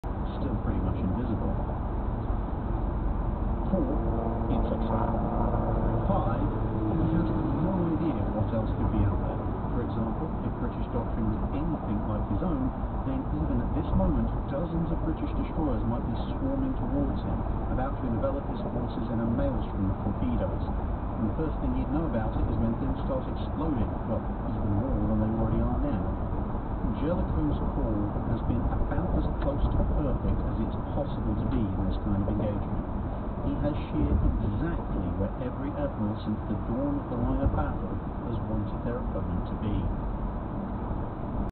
Los Angeles Interstate 5 AH1 Supercobra flyby APEMAN Dash Cam 1080P Car Camera C450 Video Sample
APEMAN Dash Cam 1080P Car Camera with 3" LCD Screen, 170° Wide Angle, G-Sensor, WDR, Loop Recording, Motion Detection, Night Vision C450  video sample.
buzzed by AH1 Super cobra
Interstate 5 in Los Angeles